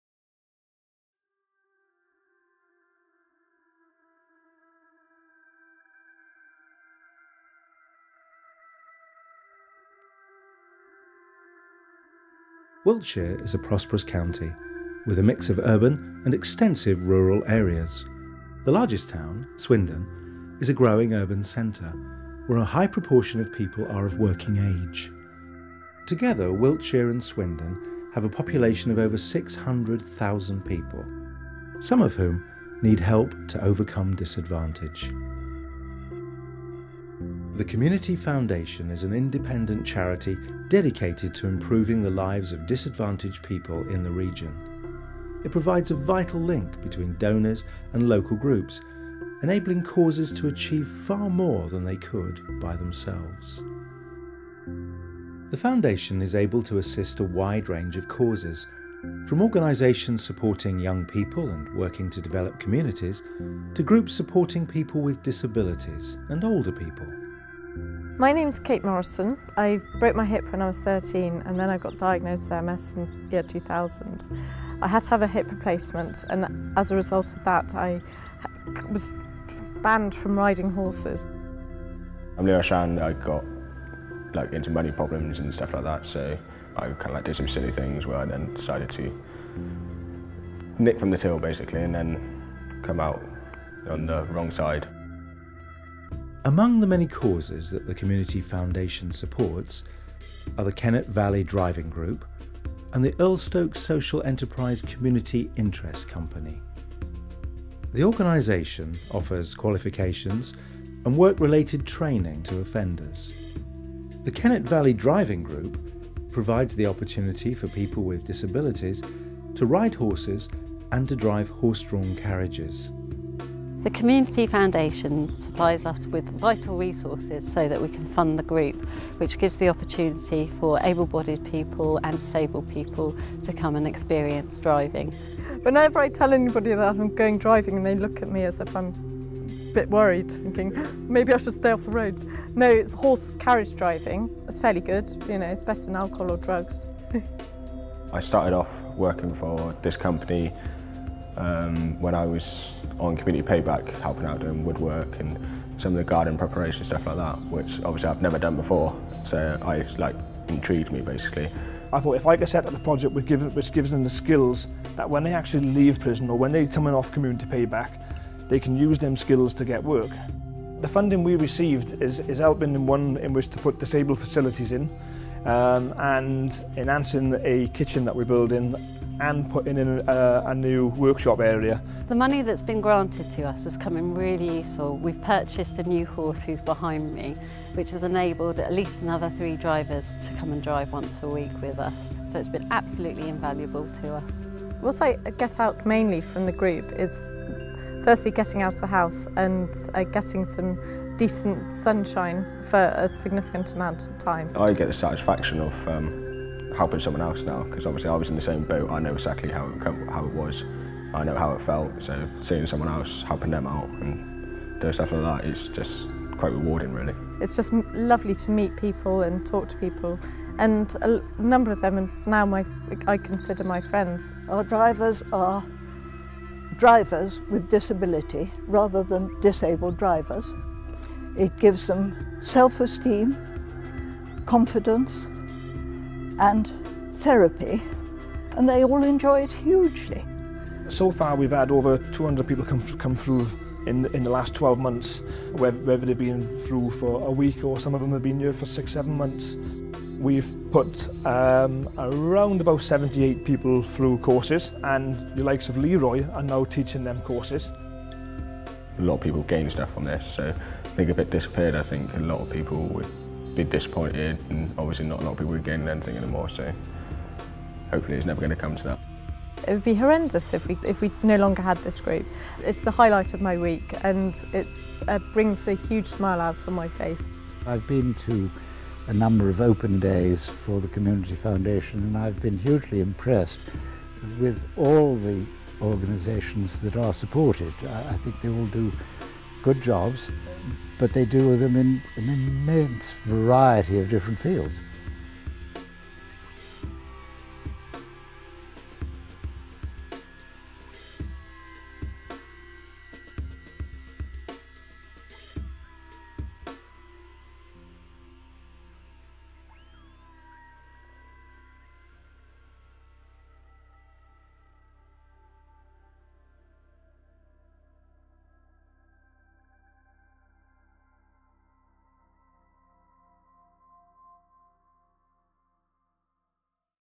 Together Making Our Community Better
Outlining two of the groups that The Community Foundation supports this short promotional film gives a flavour of the diverse range of casues that can be supported via a Community Foundation.   Produced by Level Films Films for The Community Foundation for Wiltshire and Swindon.